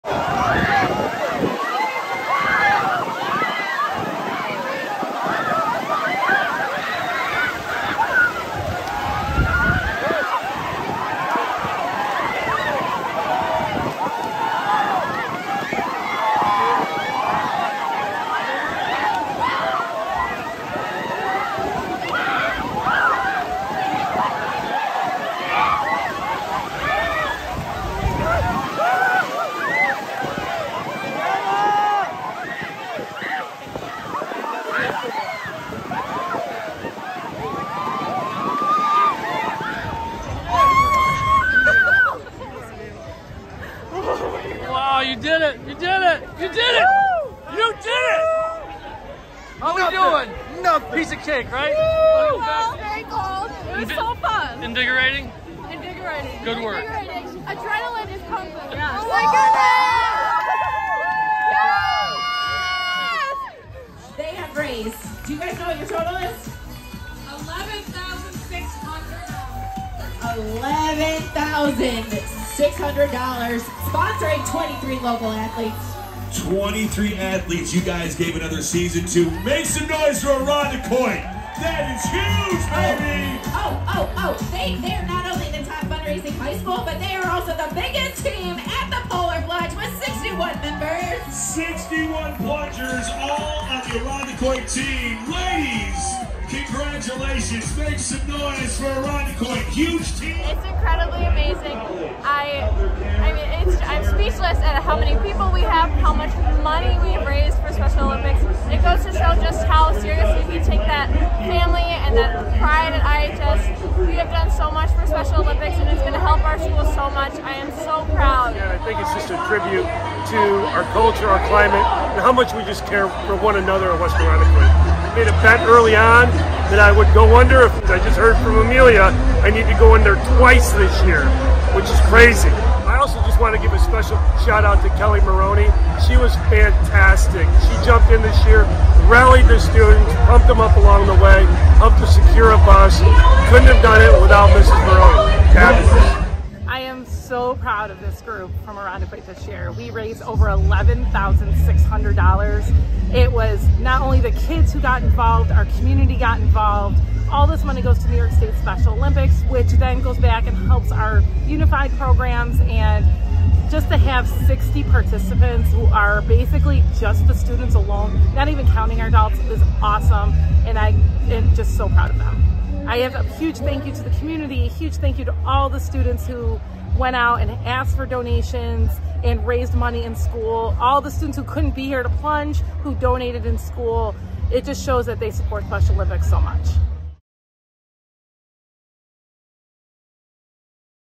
Polar Proud: We raised almost $12,000 for Special Olympics!
West Irondequoit has had faculty and students participate in Rochester’s annual Polar Plunge in the past, but Sunday was unlike any other. Our “Irondequoit Icy Eagles,” a team moniker hatched in 2023 by Superintendent and team captain Aaron Johnson, had 61 plungers into Lake Ontario – more than any other group at the event! We also won the “Cool School Challenge.” Our total of $11,600 (at the time) was the most of any local school.

The donation to Special Olympics New York has now climbed to $11,928. The event itself topped $300,000.

“I think it’s a tribute to our culture, our climate and how much we just care for one another in West Irondequoit,” Johnson said. “I also want to give a shoutout to Kelly Moroni. She was fantastic. She jumped in this year, rallied the students, pumped them up along the way. We couldn’t have done it without (her).”

Mrs. Moroni is a Physical Education teacher at Irondequoit High School, a coach for Unified Sports and an IHS graduate. “I am so proud of this group. It was not only our kids that got involved, but our community also got involved,” she said.

Johnson made good on his promise to students. He pledged to go under water if they raised more than $5,000 and he did so with a shallow (yet quick) dive after wading into the frigid waters. “All for a great cause,” he said.

Thank you students, staff and all who donated! The “awards” are nice. The difference the dollars will make to help Special Olympics athletes and families is the real reward!